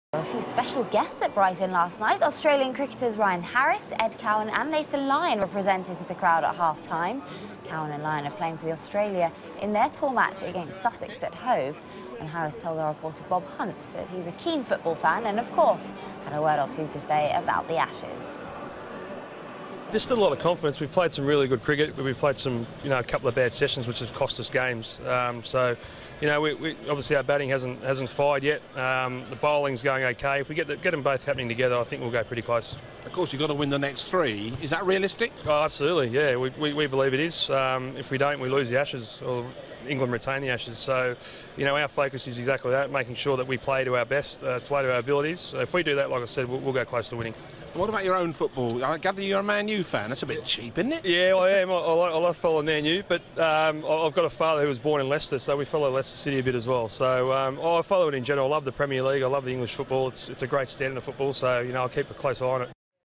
Harris to pitch in
There were a few special guests at Brighton last night. Australian cricketers Ryan Harris, Ed Cowan and Nathan Lyon were presented to the crowd at half time.